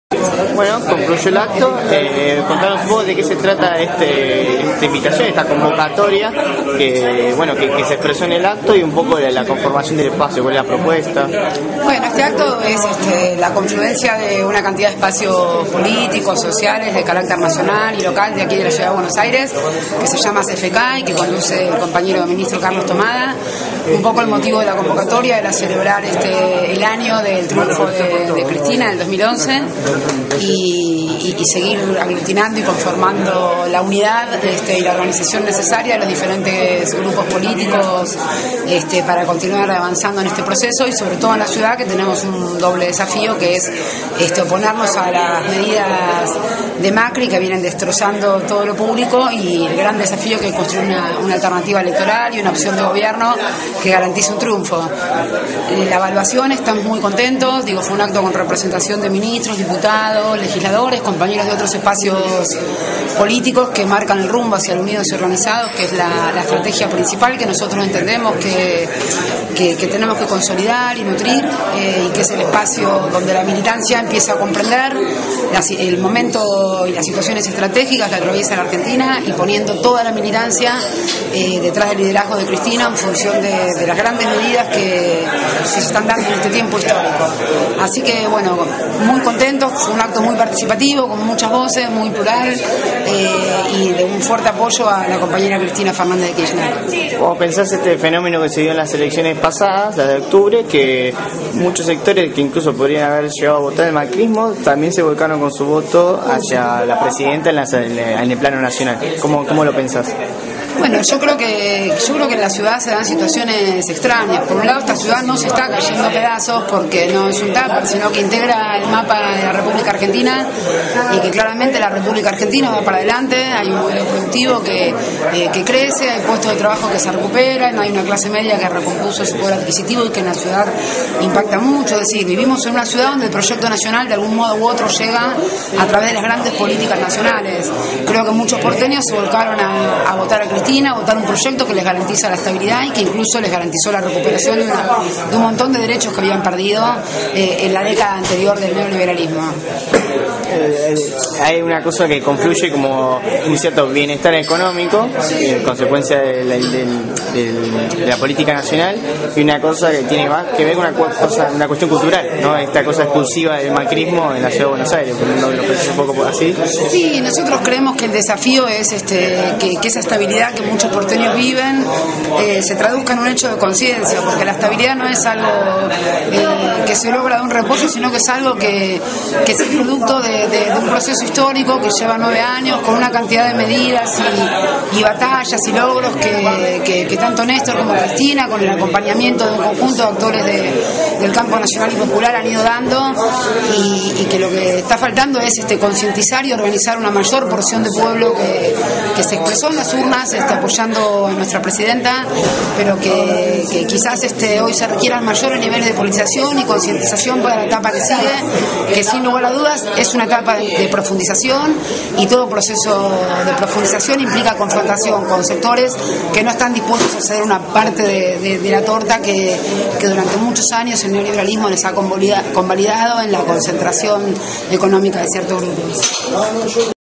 Lorena Pokoik
Acto convocatoria por un Frente Kirchnerista en CABA